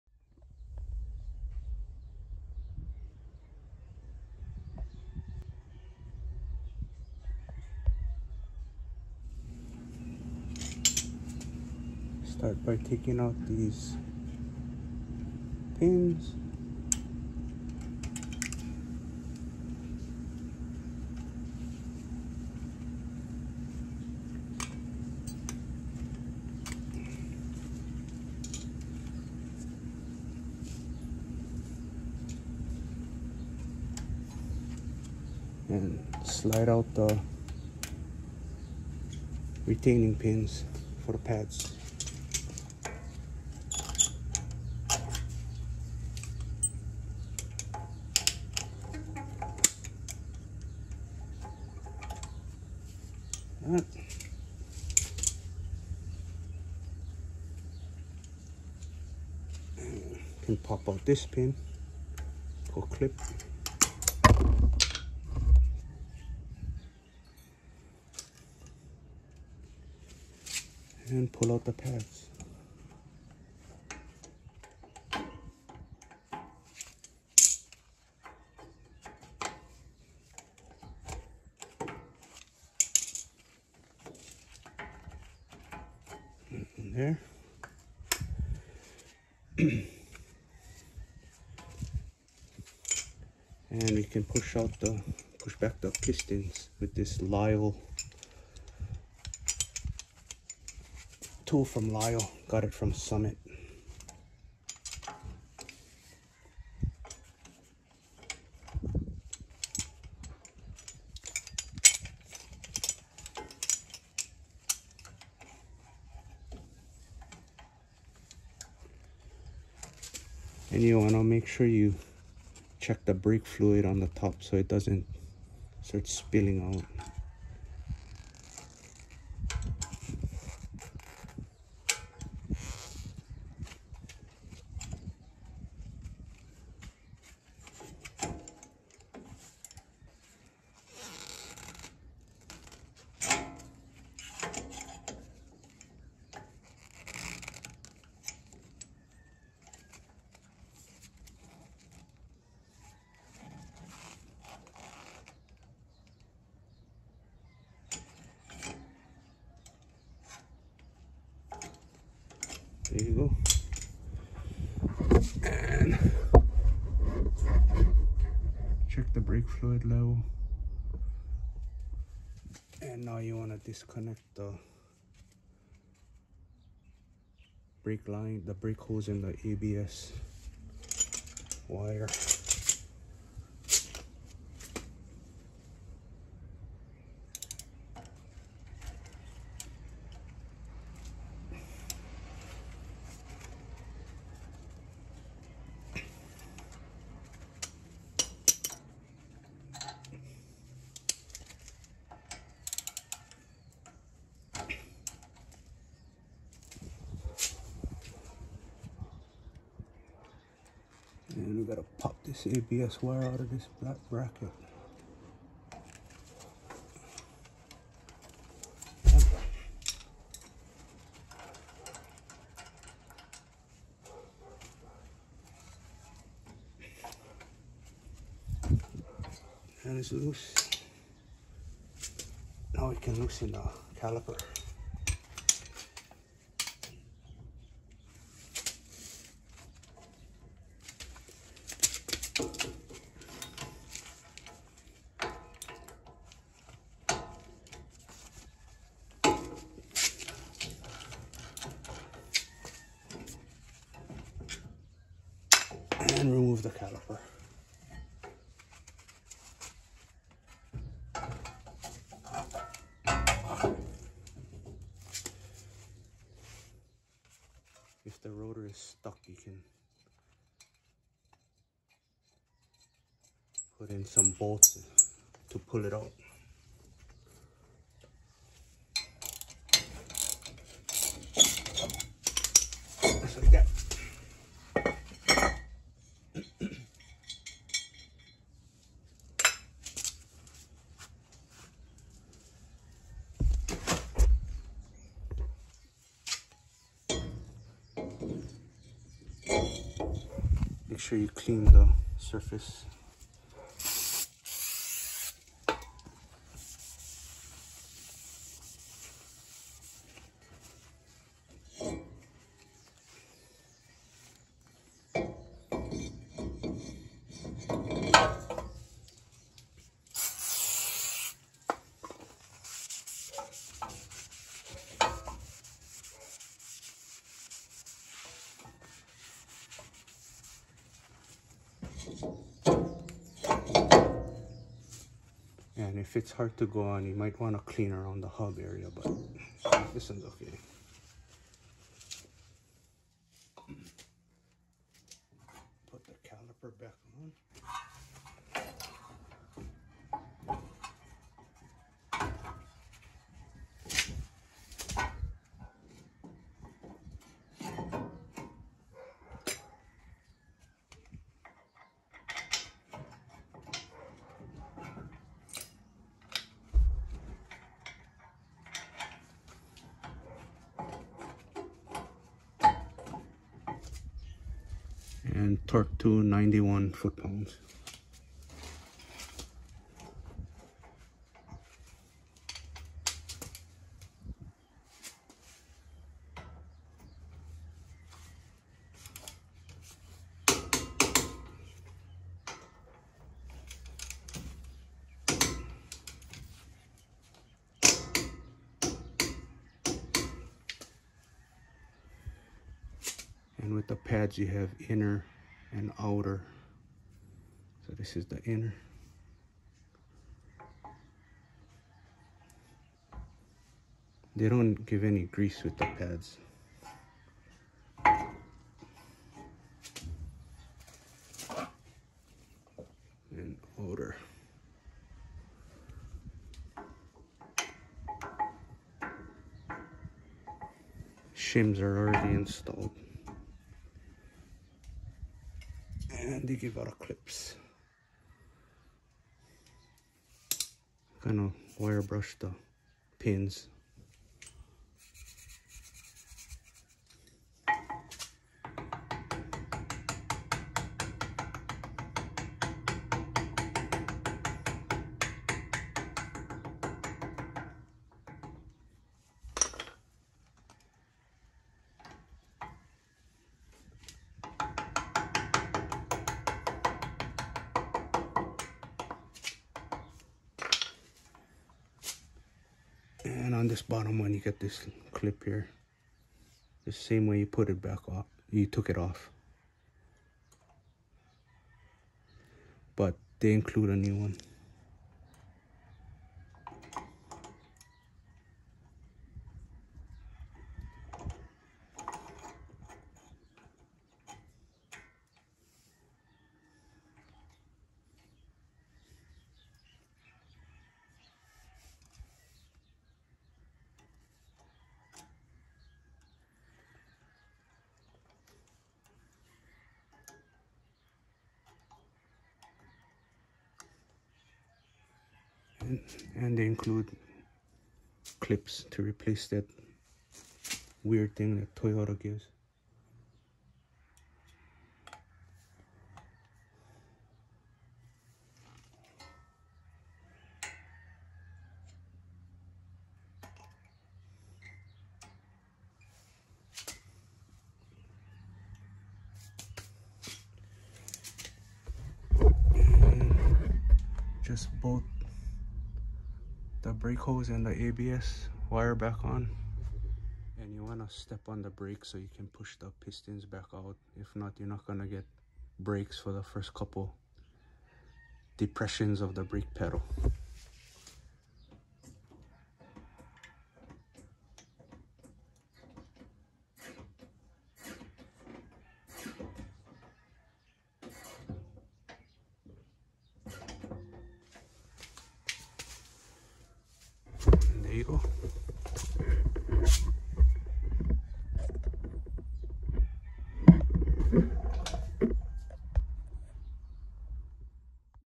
How to do a Front Brake Job - 3rd Gen Toyota Tacoma
Replacing the front brake pads & rotors on a 2016 Toyota Tacoma TRD Off Road, V6, 4X4.  Napa Adaptive One brake pads (Part number AD7877) & Ultra Premium polymer coated rotors (Part number 880670CR).  Lisle 29100 Quick Quad pad spreader.  Filmed with an iphone 11 in a Joby Gorillapod.